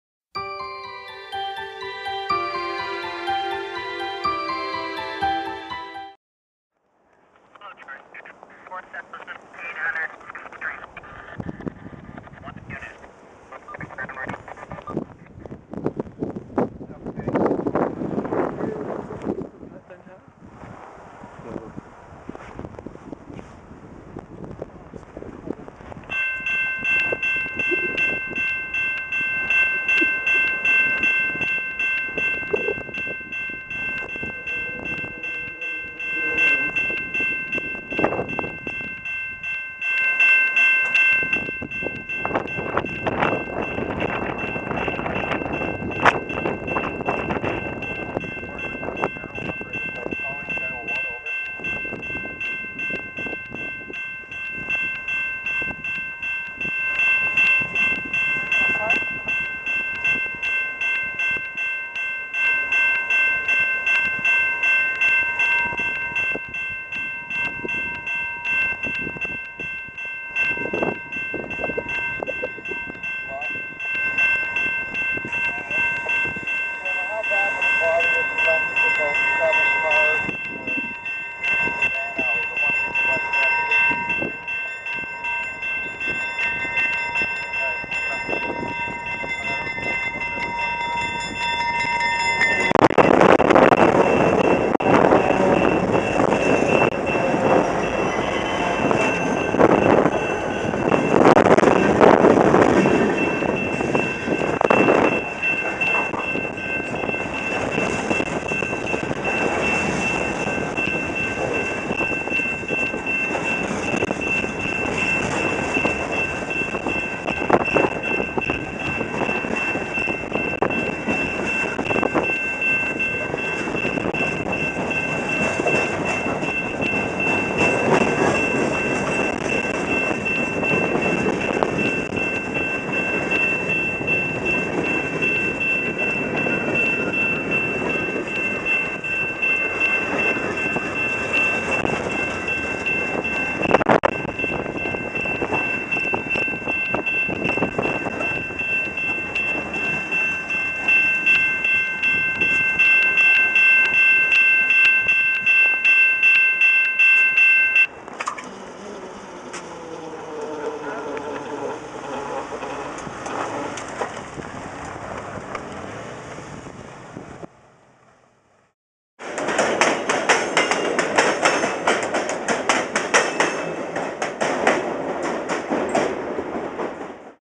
CP Train Crosses Third St.
A Canadian Pacific freight train crosses Third Street in London, Ontario, approaching the Quebec St. Yard.